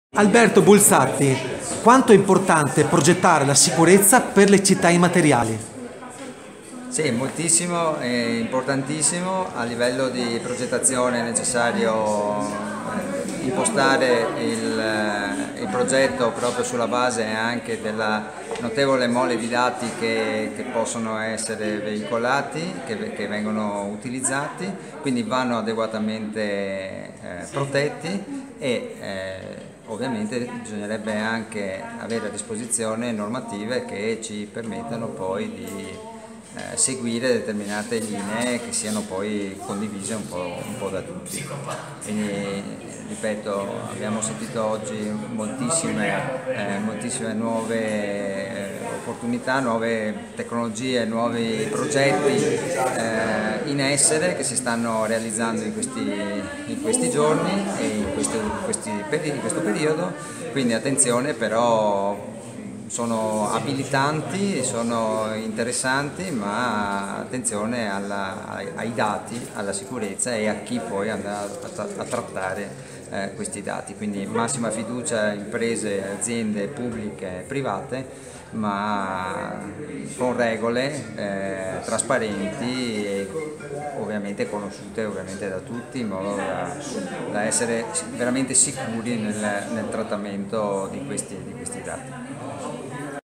Alberto Bulzatti e la Città Immateriale - Convegno Collegio Ingegneri Venezia
LA CITTÀ IMMATERIALE
Infrastrutture digitali, algoritmi e stili di vita

Convegno del 28 gennaio 2020 organizzato dal Collegio Ingegneri Venezia presso la sede dell’Ordine degli IngegneriVenezia. Focus sulle profonde trasformazioni generate dalle Tecnologie dell’Informazione e Comunicazione sulle nostre città.

Insostituibili compagne della nostra quotidianità, l’intelligenza artificiale, la realtà aumentata e le altre tecnologie sono diventate parte integrante di come viviamo e progettiamo le nostre città.

All'evento, curato da Fulvio Caputo e Corrado Poli, sono intervenuti esponenti di aziende private che realizzano la “città immateriale”, rappresentanti di laboratori di ricerca che studiano le nuove applicazioni da porre nell’“infosfera” e studiosi che esaminano l’impatto di queste nuove tecnologie sulle nostre società e sui nostri stili di vita.